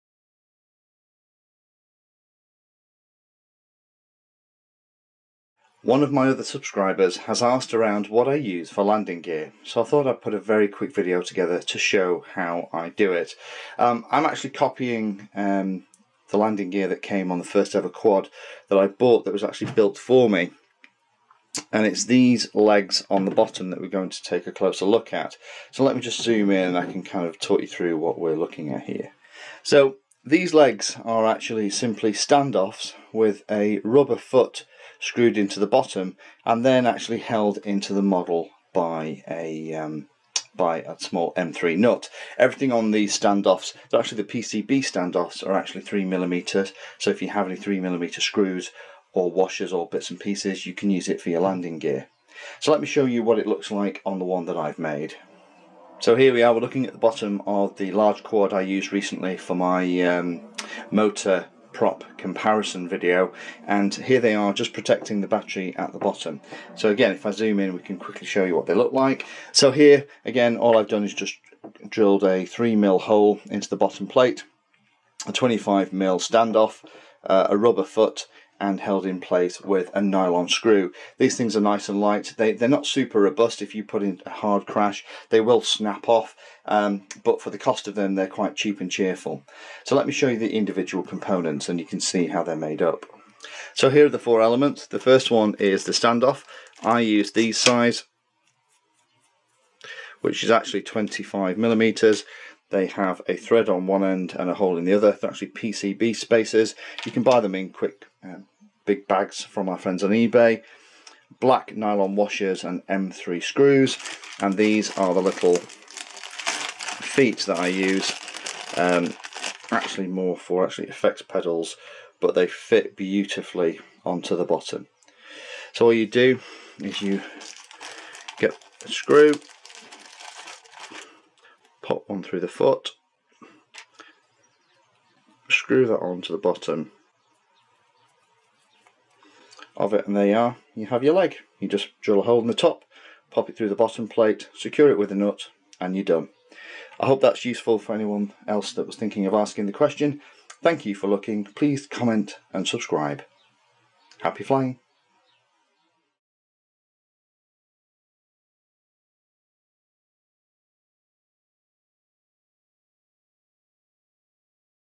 Multirotor landing gear - what I use and how I make them
This is to answer one of my subscribers and shows how I make landing gear for my quad/multi-rotors. There are many ways to do this, this just happens to be mine. Each leg weighs 1.8 grams.

Happy flying!